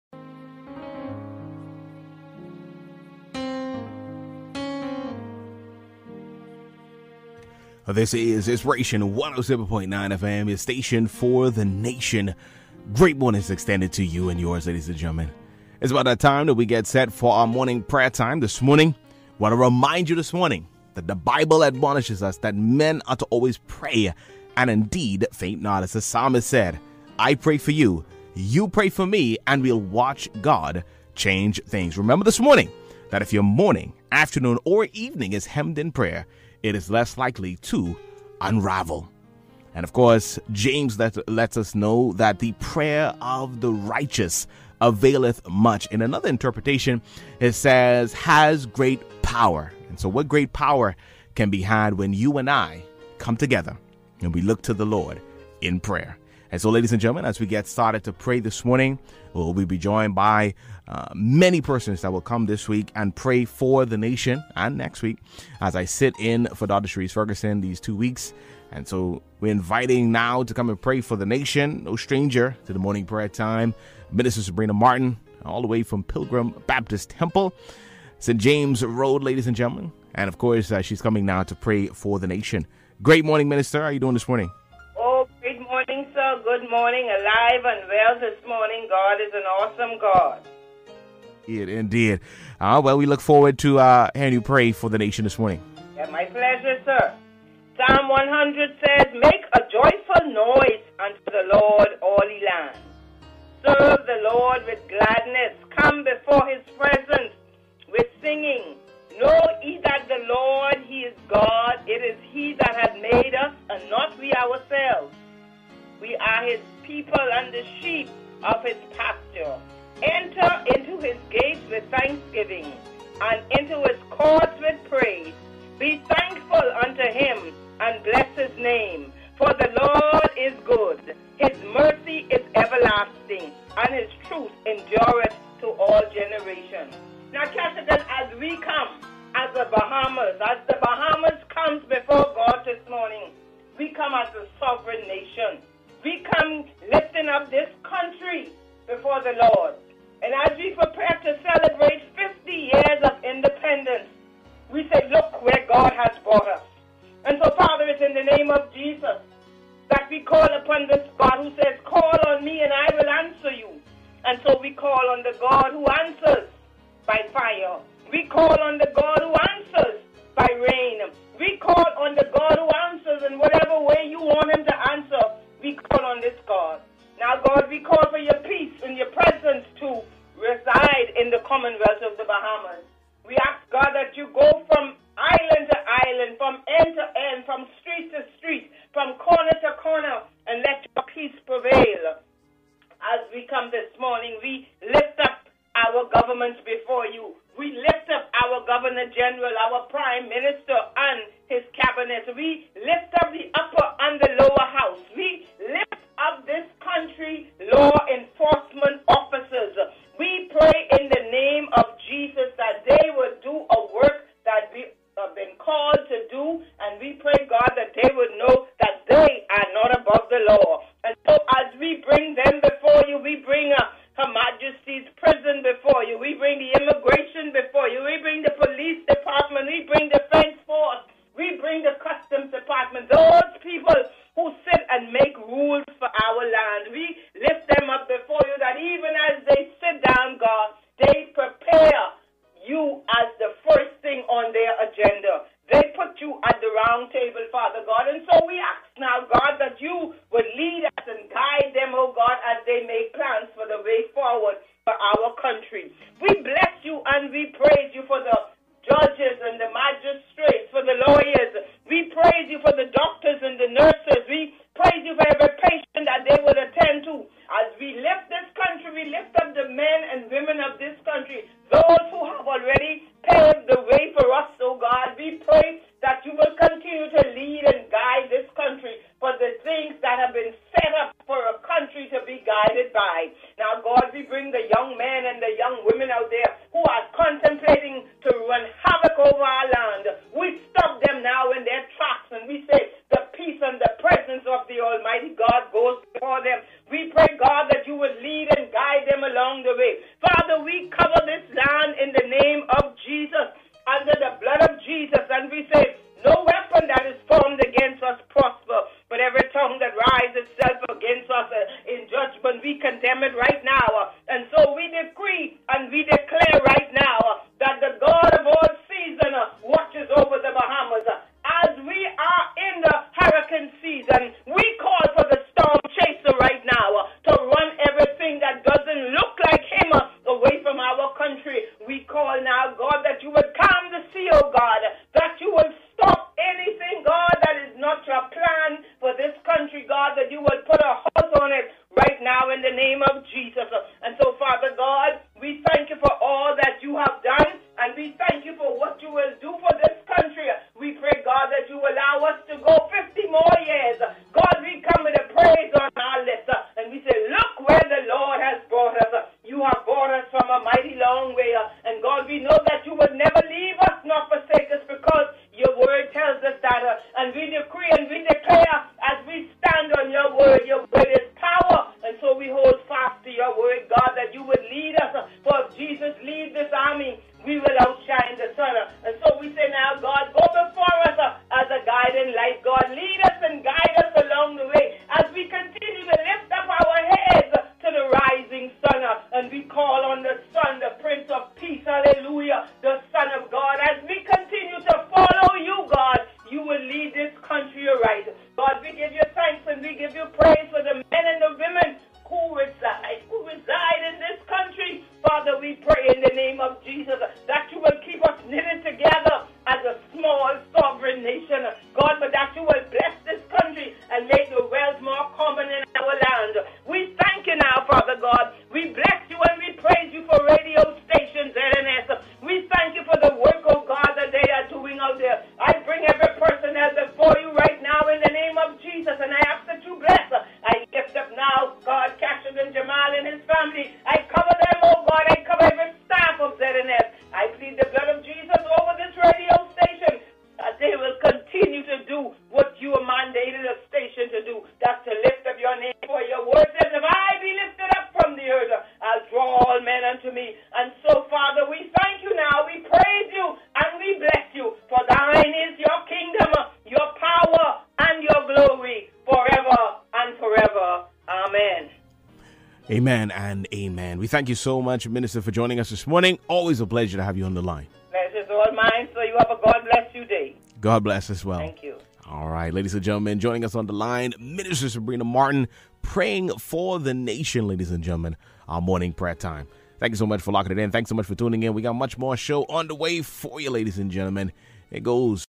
MORNING PRAYER TIME MIN. SABRINA MARTIN | The Keshagen Adderley Podcast
Let us Pray! This week we continue to cover our Nation in Prayer. I'll be join by Pastors across the country each morning at 6:30am! As we pray for Peace and Health and as the lord would lead.

Joining us is Min. Sabrina Martin, Nassau, Bahamas.

2 Chronicles 7:13-14 New International Version (NIV) 13 “When I shut up the heavens so that there is no rain, or command locusts to devour the land or send a plague among my people, 14 if my people, who are called by my name, will humble themselves and pray and seek my face and turn from their wicked ways, then I will hear from heaven, and I will forgive their sin and will heal their land.